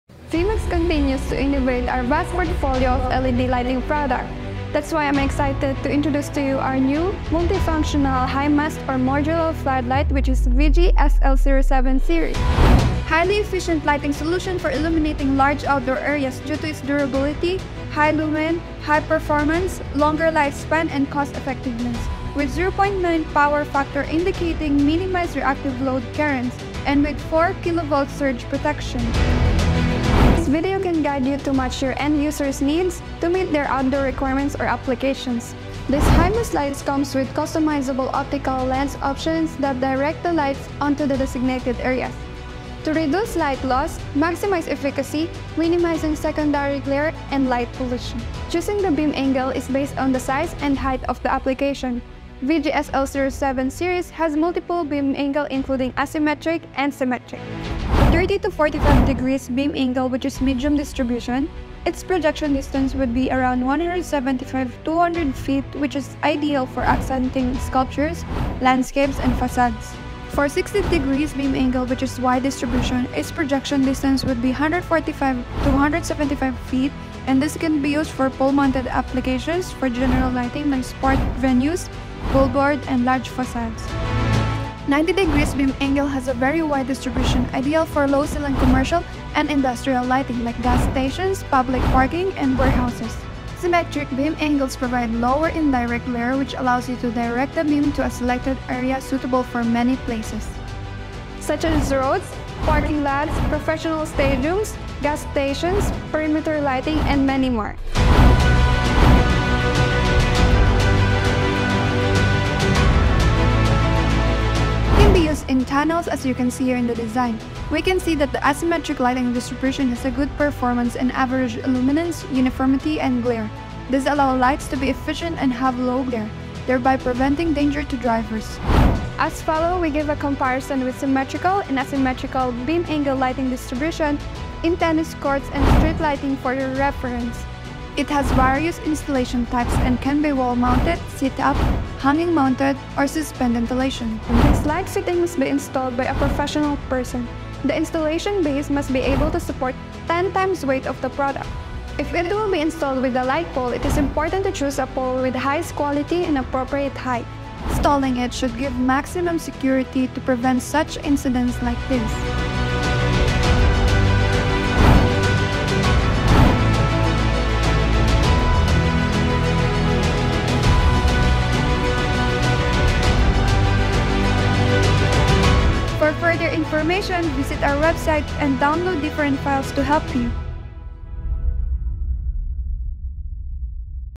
Modular LED High Mast l LED Stadium Floodlight
VMAX continues to innovate and enhance our vast portfolio of LED lighting products. That's why we're excited to introduce to you our new multifunctional outdoor high mast or modular flood light which is VG-SL07 Series, this video can guide you to match your end user's needs to meet their outdoor requirements.

Highly efficient Lighting Solution for Illuminating Large Outdoor Areas due to its durability, high lumen, high performance, longer lifespan, and cost-effectiveness.